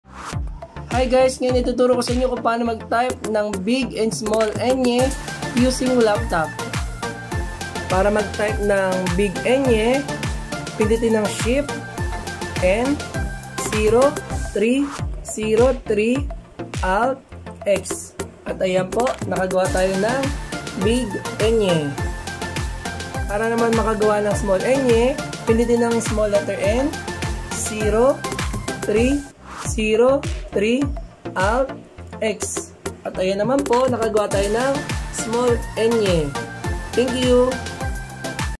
How to type Enye in Laptop. BIG and small Enye (Ññ) using Laptop.
Watch this 30secs tutorial video on How to type BIG AND small Enye (Ñ and ñ) using Laptop.